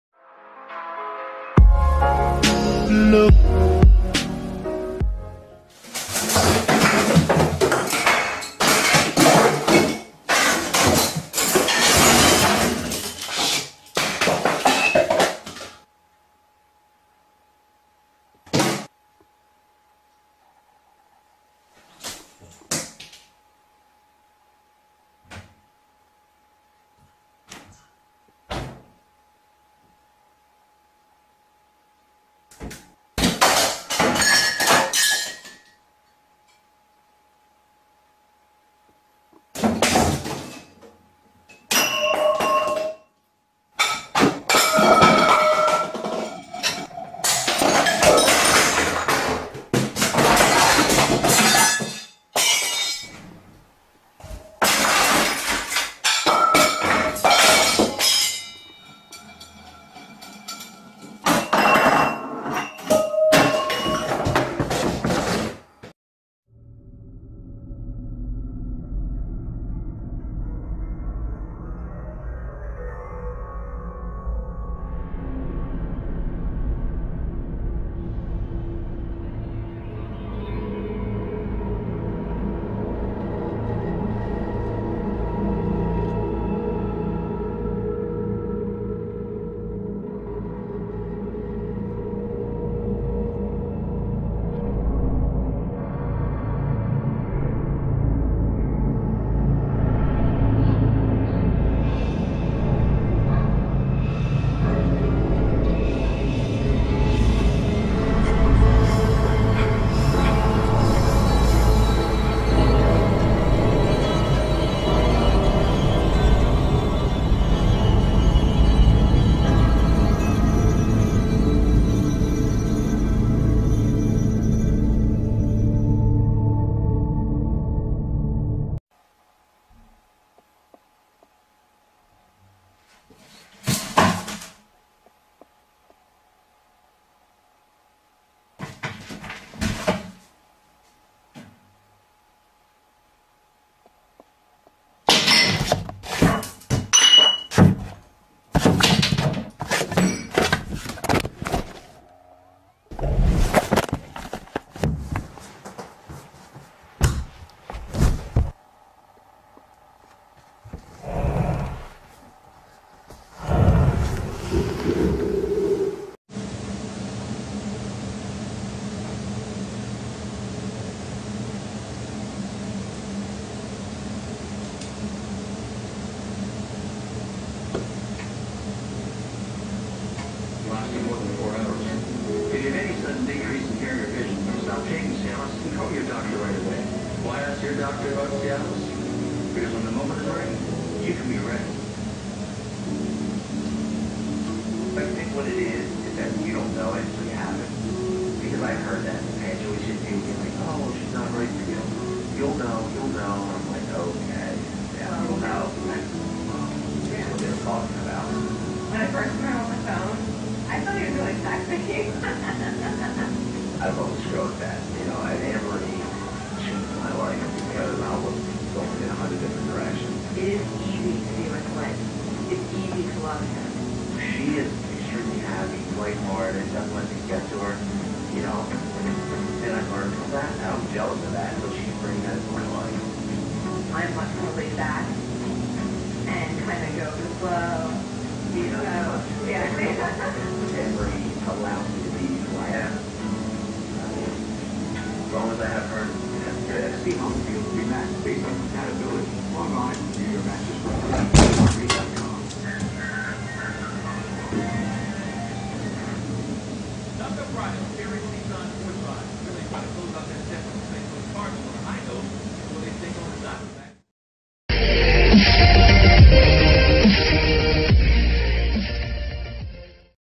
Cctv paranormal activity video - Cctv World
This video is real no editing or fake.
This footage consist paranormal activities at night in different places.
If you have not watched the complete video please watch it again.

If you like please share to more people.